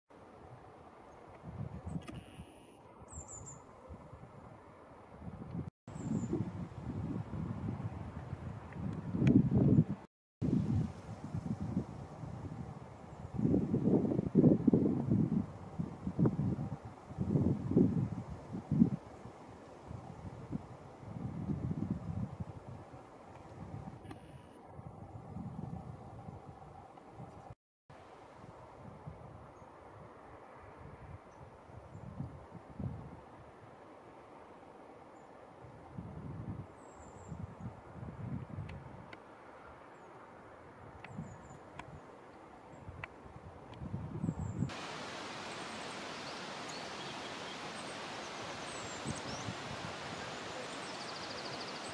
goldcrest at swans park
goldcrest on a branch at swans park, buncrana, finally managed to get a decent video of this tiny bird